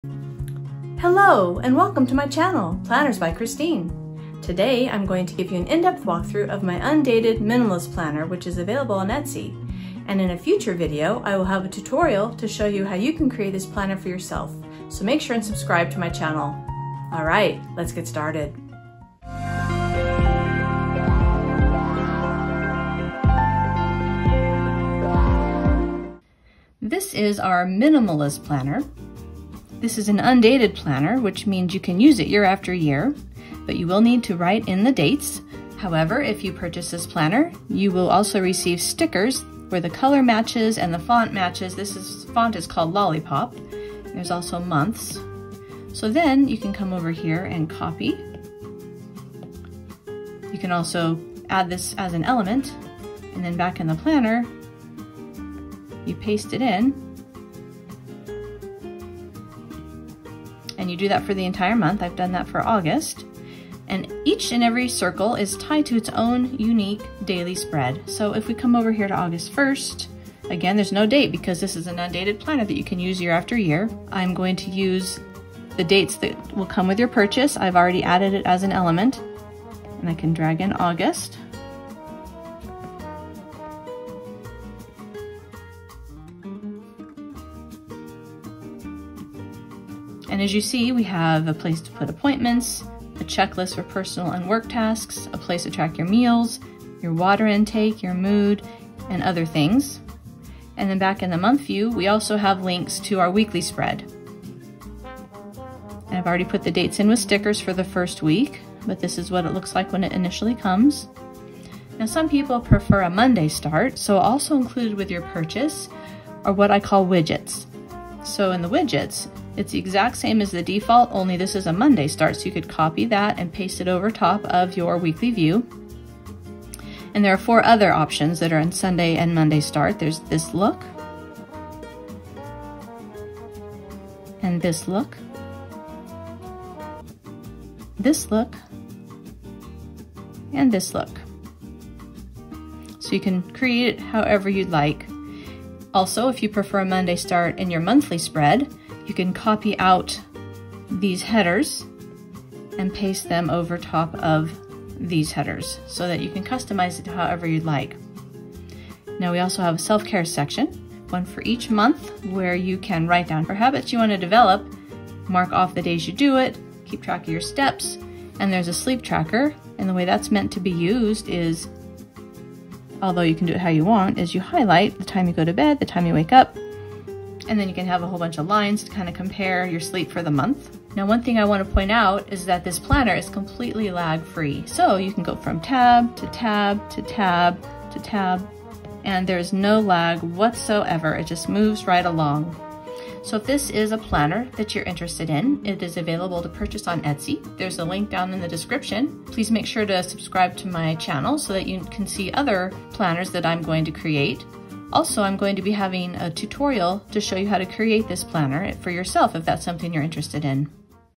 Best Minimalist Digital Planner | Undated Minimalist Digital Planner Walk-through
The Undated Minimalist Digital Planner is a simple, yet powerful planner that will help you get--and keep!--your life organized. This is an undated planner, which means it can be used year after year. The Minimalist Digital Planner is completely lag-free.

This walk-through will show you the planner in more detail than you can see on Etsy.

iPad Air 4
Apple Pencil
Goodnotes 5 app